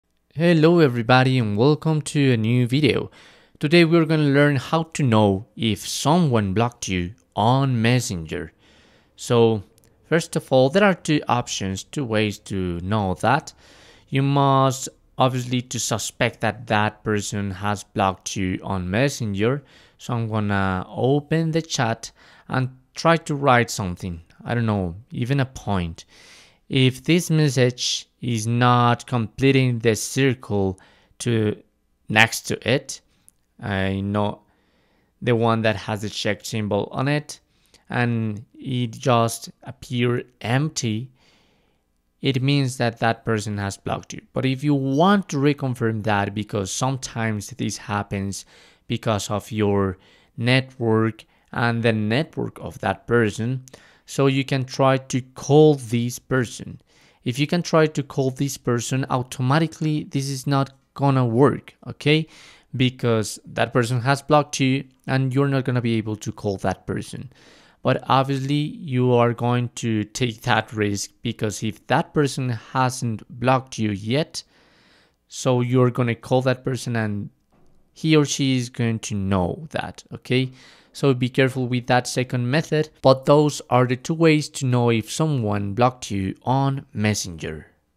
How to Know if You were Muted on Messenger in 2023
In this video I'll show you How to know if you were muted on Messenger in 2023. Thw method is very simple and clearly described in the video. Follow all of the steps and succesfully solve your problem!